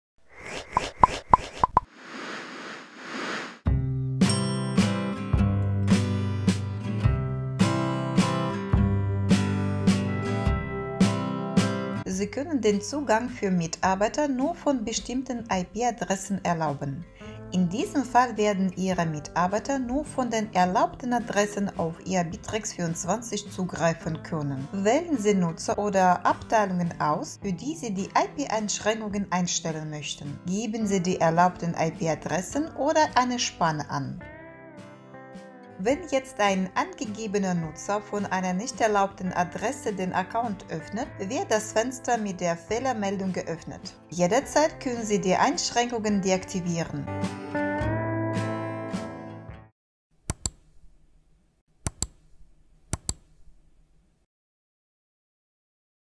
IP-Einschränkungen | Zusammenarbeit - Bitrix24
Man kann auf Bitrix24 von allen PCs und mobilen Anwendungen zugreifen. Sie können aber, wenn notwendig, den Zugang für Mitarbeitenden nur von bestimmen IP-Adressen erlauben. In diesem Fall werden Ihre Mitarbeiter nur von den erlaubten Adressen auf Ihr Bitrix24 zugreifen können, sonst wird der Zugriff für sie verweigert.